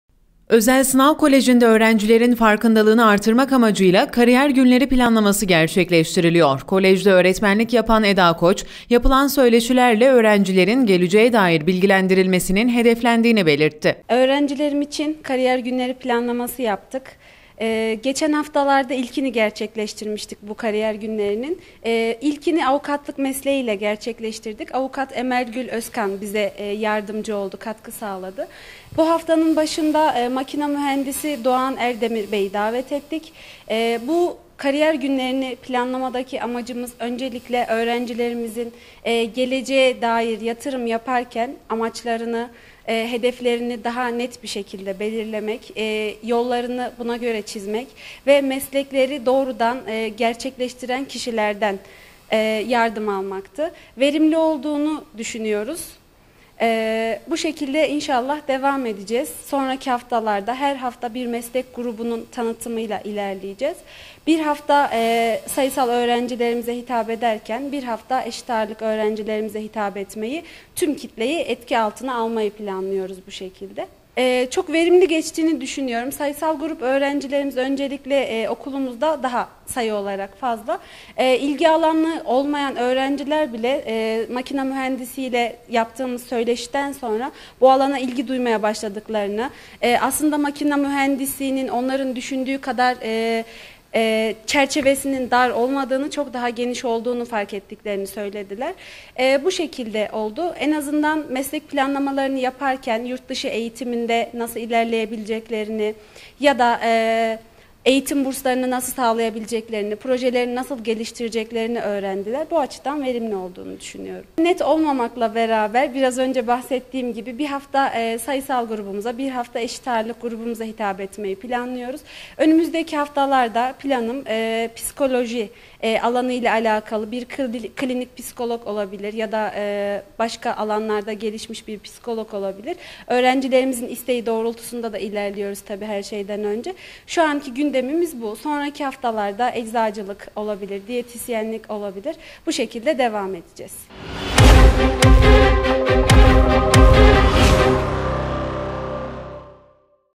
Sınav Koleji’nde Kariyer Günleri
KAYSERİ (1HA) – Özel Sınav Koleji’nde öğrencilerin farkındalığını arttırmak amacıyla ‘Kariyer Günleri’ planlaması gerçekleştiriliyor. Kolejde öğretmenlik yapan Eda Koç, yapılan söyleşilerle öğrencilerin geleceğe dair bilgilendirilmesinin hedeflendiğini belirtti.
Özel Sınav Koleji tarafından öğrencilerin bilgilendirilmesi amacıyla ‘Kariyer Günleri’ başlatıldı. Birçok meslek gruplarının öğrencilerle bir araya gelerek onlara eğitim vereceğini söyleyen Türk Dili ve Edebiyatı Öğretmeni Eda Koç, “Kariyer günleriniz planlamadaki amacımız öncelikle öğrencilerimizin geleceğe dair yatırım yaparken amaçlarını, hedeflerini daha net bir şekilde belirlemek ve yollarını buna göre çizmektir” diye konuştu.
 ‘MESLEKLERİ DOĞRUDAN GERÇEKLEŞTİREN KİŞİLERDEN YARDIM ALMALARI ÇOK ÖNEMLİ’
Kariyer günlerinin birçok meslek grubuyla yapılmasının planlandığının altını çizen Koç, “Öğrencilerimiz için kariyer günleri planlaması yaptık. Geçen hafta da ilk eğitimi avukatlık mesleği üzerine gerçekleştirdik. Bu haftanın başında ise Makine Mühendisliği eğitimimiz oldu. Kariyer günleriniz planlamadaki amacımız öncelikle öğrencilerimizin geleceğe dair yatırım yaparken amaçlarını, hedeflerini daha net bir şekilde belirlemek ve yollarını buna göre çizmektir. Meslekleri doğrudan gerçekleştiren kişilerden yardım almaları çok önemli ve verimli olduğunu düşünüyoruz. Her hafta bir meslek grubuyla bir araya gelmeyi planlıyoruz” ifadelerini kullandı.
Öğrencilerin geleceğe yönelik bilinçlendirilmesi konusunda hassas olduklarını ifade eden Koç sözlerini şöyle sürdürdü: “Sayısal grup öğrencilerimizin sayısı daha fazla. 11. Sınıf öğrencilerimiz ve eşit ağırlık öğrencilerimiz bile geçen hafta yapmış olduğumuz söyleşiden sonra bu alana ilgi duymaya başladıklarını söyledi. Makine mühendisliğinin düşündükleri kadar çerçevesinin dar değil geniş olduğunu fark ettiklerini söylediler. En azından meslek planlamalarını yaparken yurtdışı eğitiminde nasıl ilerleyebileceklerini ya da eğitim burslarını nasıl sağlayabileceklerini, projeleri nasıl geliştireceklerini öğrendiler ve bu açıdan verimli olduğunu düşünüyorum.”